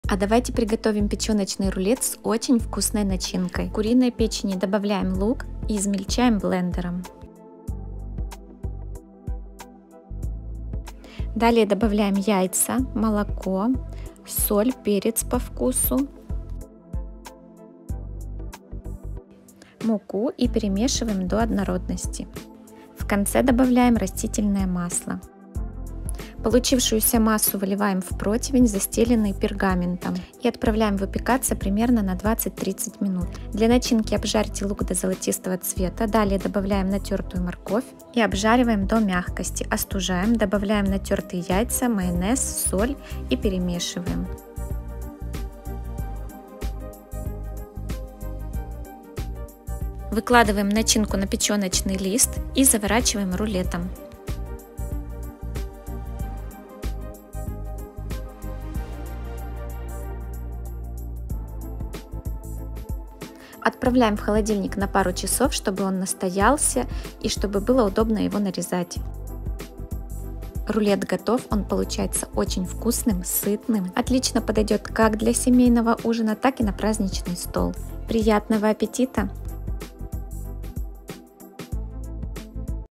Печеночный рулет с очень вкусной начинкой
✔️Ингредиенты:
500 грамм куриной печени
3 яйца
150 мл. молока
1 лук
5 ст.лож муки
2 ст.лож растительного масла
Соль, перец по вкусу
✔️Для начинки:
1 морковь
1 лук
5 шт яиц
Майонез
✔️Способ приготовления:
Куриную печень промойте, добавьте очищенный лук и пробейте блендером.
Далее добавляем яйца, молоко, соль, перец, муку и перемешиваем до однородной. В конце добавляем растительное масло.
Противень застилаем пергаментом и выливаем получившуюся массу.
Отправляем в предварительно разогретую духовку до 200 градусов на 20-30 минут.
Для начинки: обжариваем лук до золотистого цвета, добавляем натертую морковь и обжариваем до мягкости.
Остужаем, добавляем варёные натертые на мелкой тёрке яйца, майонез, соль, перец и перемешиваем.
Выкладываем начинку на печёночный лист и заворачиваем рулетом.
Отправляем на пару часов в холодильник, чтоб он настоялся и удобнее было его нарезать.
Нарезаем рулет на кусочки шириной примерно 4 см.
🍽Приятного аппетита 💝